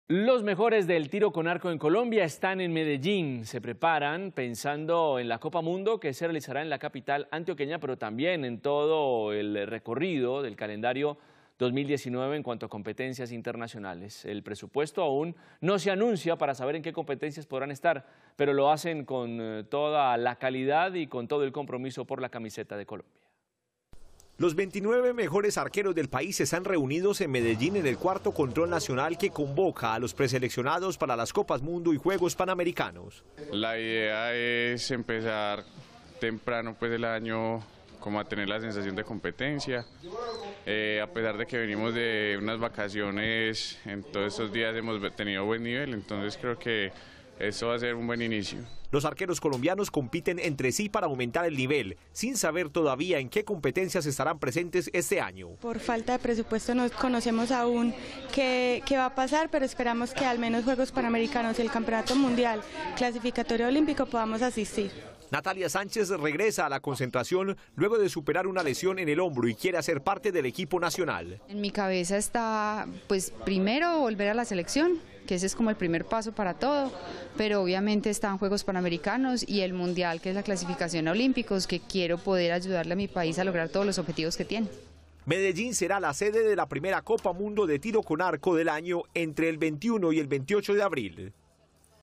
Los mejores 29 arqueros del país están reunidos en Medellín [Noticias] - Telemedellín
Los mejores 29 arqueros del país están reunidos en Medellín en el Cuarto Control Nacional que reúne a los preseleccionados para las Copas Mundo y Juegos Panamericanos.